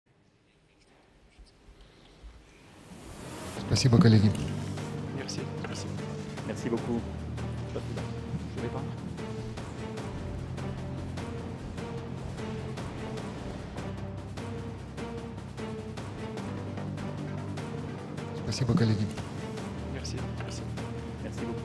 شاهد.. بوتين "يخرق" البروتوكول ويلمح لـ "ماكرون" بيده للحاق به
شاهد بالفيديو.. بوتين "يخرق" البروتوكول الرسمي ويغادر قاعة مؤتمره مع ماكرون ملوحاً بـ "إشارة" كي يلحق به